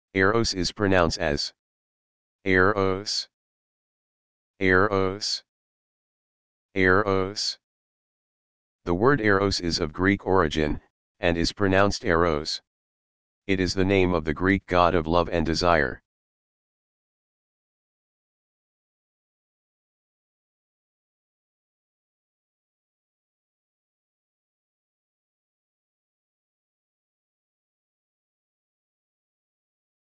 How To Pronounce Eros
The word "Eros" is of Greek origin, and is pronounced "eh-rohs". It is the name of the Greek god of love and desire.